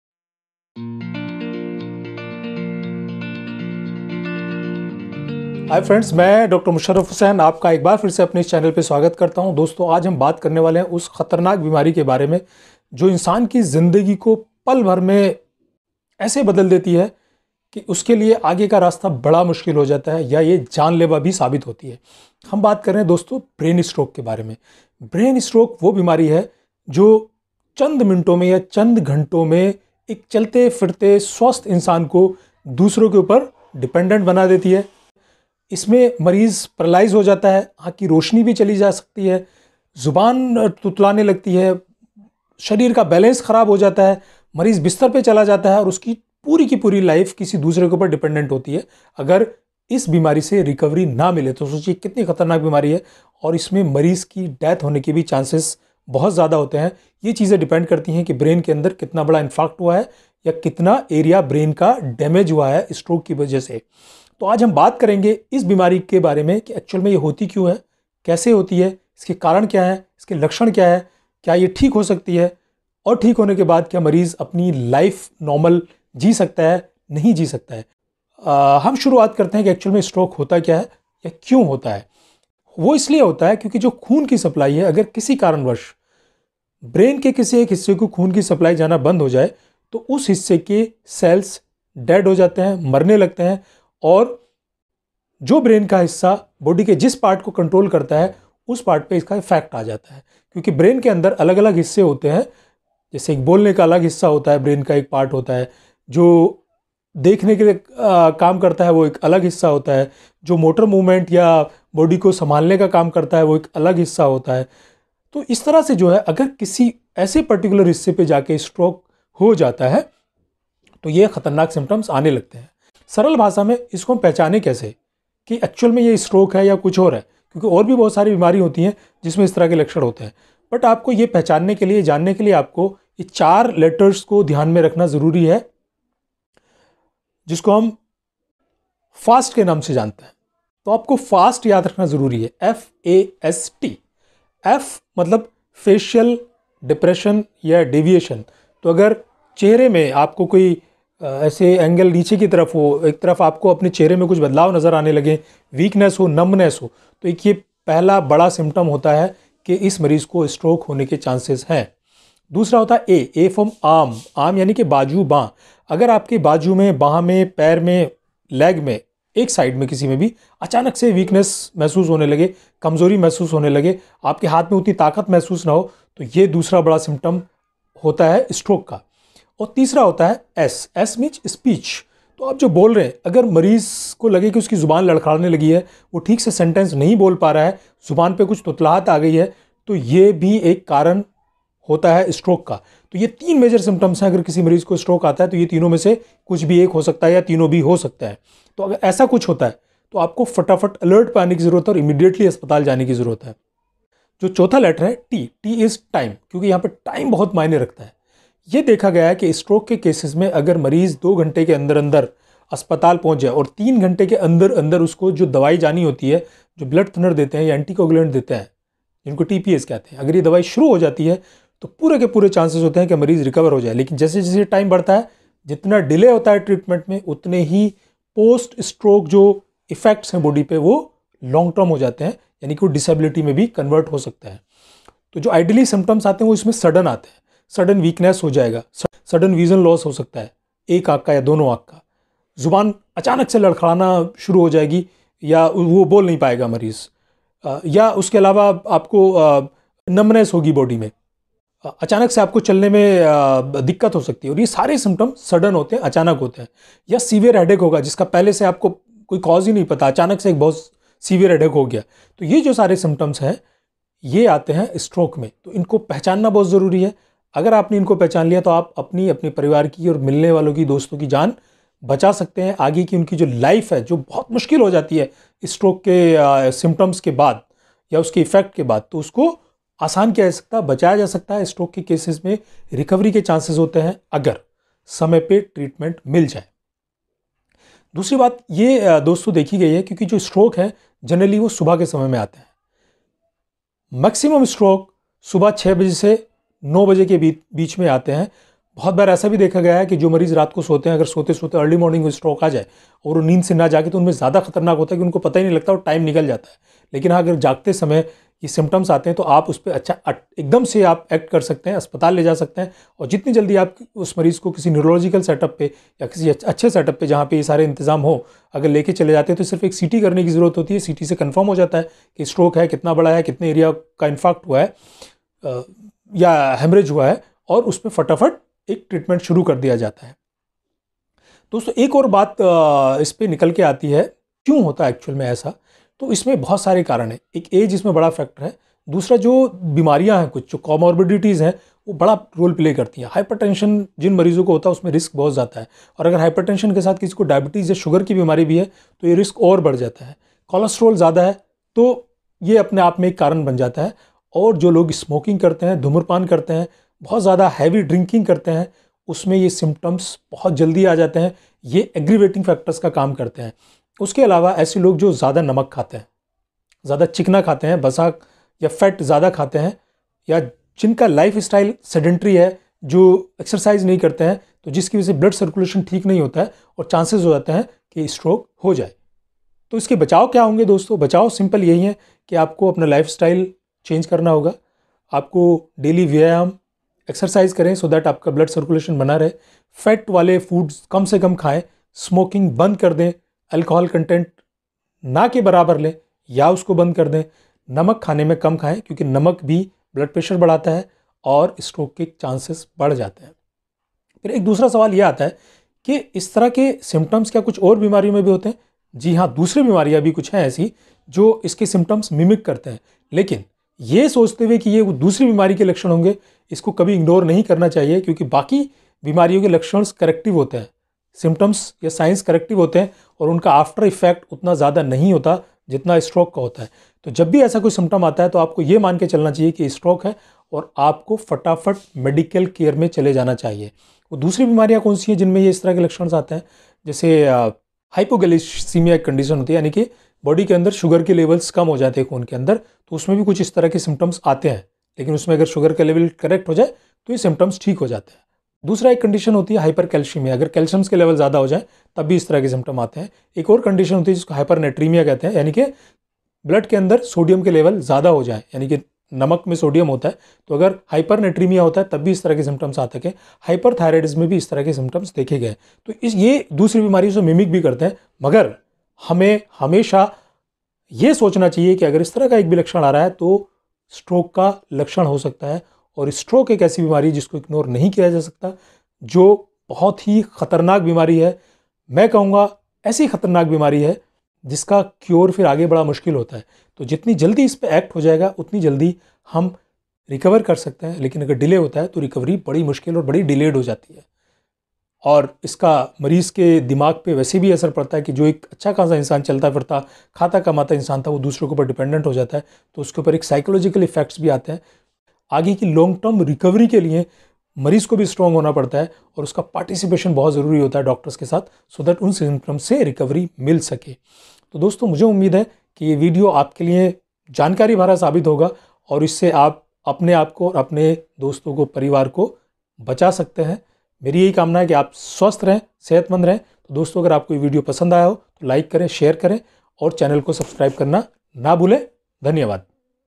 Brain Stroke से कैसे बचें | ब्रेन स्ट्रोक के कारण, बचाव व इलाज | FAST | Dr Musharraf Husain
Brain Stroke से कैसे बचें | ब्रेन स्ट्रोक के कारण, बचाव व इलाज | FAST
Dr Musharraf Husain has briefed about How To Prevent Brain Stroke, its causes, symptoms, prevention and treatment in this video. How brain stroke change the life in minutes and the impact on daily living and on the life. Brain stroke is most dangerous medical condition which is a biggest culprit for paralysis, disabilities even death in most of paralyzed cases. Common symptoms of brain stroke are Facial dropping, arms weakness and slurring of speech, time plays an important role in treatment and recovery from symptoms. FAST word is very important to remember and to identify the stroke symptoms.  This video tells about How can we prevent strokes and to identify the early symptoms before it become a danger for life.

What causes brain strokes, the Definition of brain stroke and the relationship between lifestyle and dietary habits.

What are the predisposing and risk factors and how can we prevent us by some modifications in lifestyle and dietary habits.